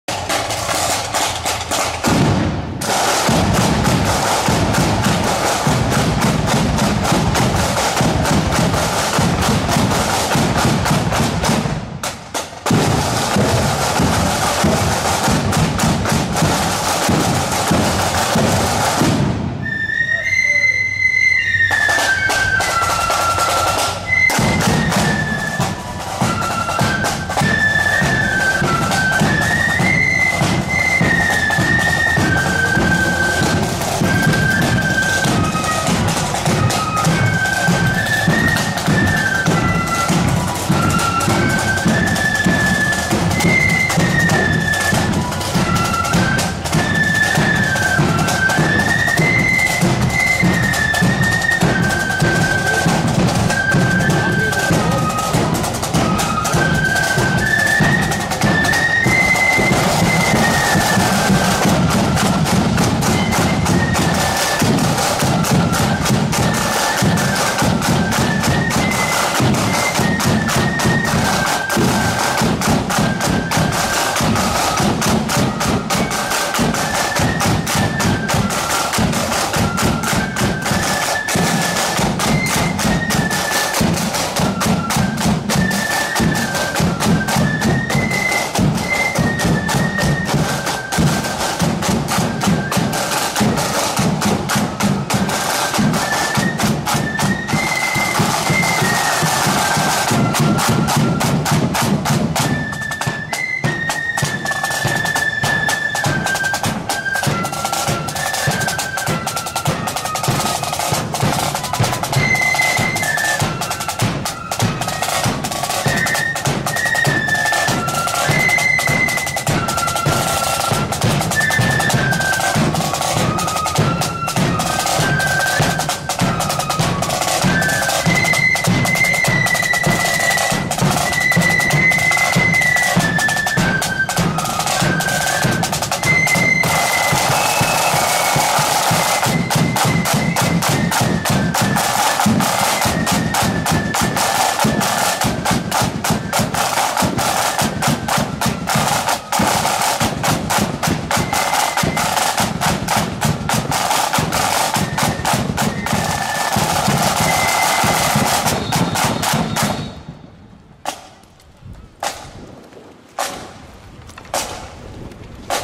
Loyal Sons Of Benagh (No.7) @ Their Own Parade ~ Newry ~ 21/04/23 (4K)
Loyal Sons Of Benagh Flute Band parading through Newry at the start of the night at their own annual parade held on Friday 21/04/2023.